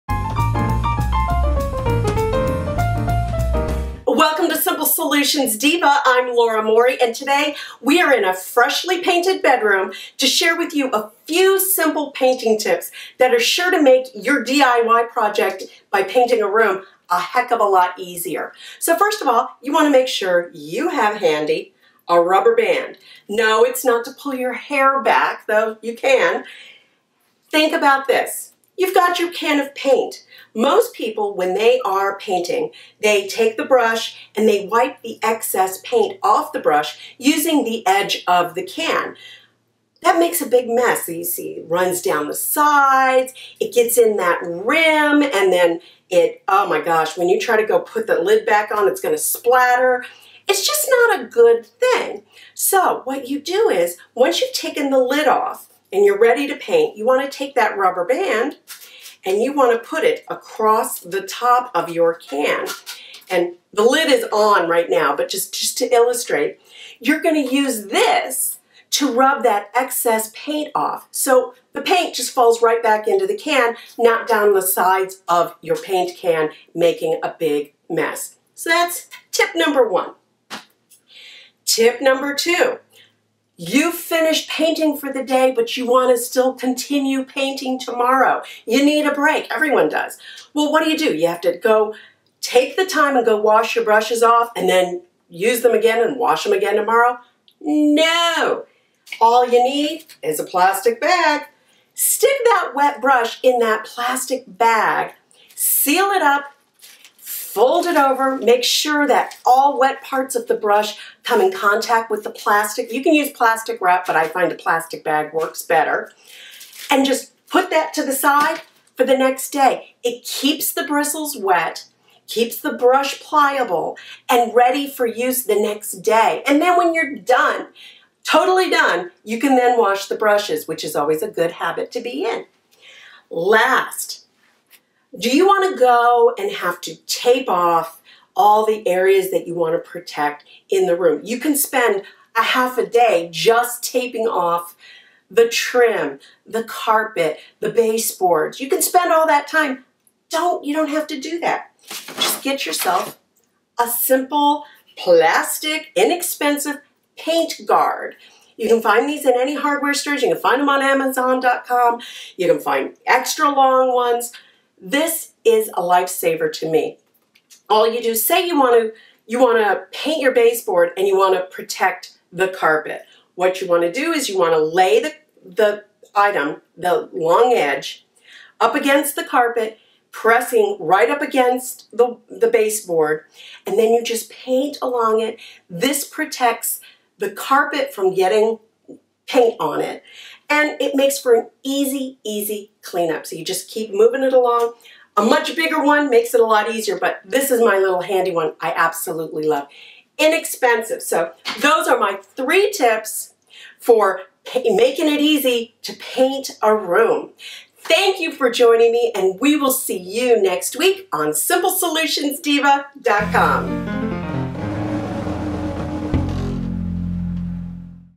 SimpleSolutionsDiva.com: Simple Painting Tips
Join Laura Morey, your Simple Solutions Diva, as she shares some simple tips to make DIY painting projects a whole lot easier! For more simple tips, DIY projects and more,